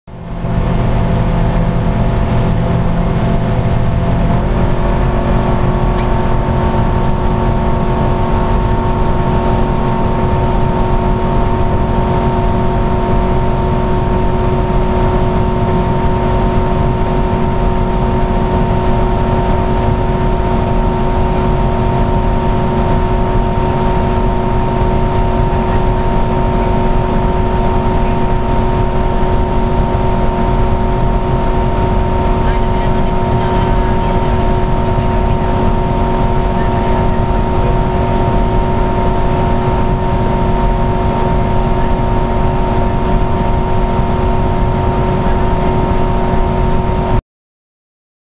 Boat
On the boat from Ushuaïa, Argentina to Puerto Navarino, Chile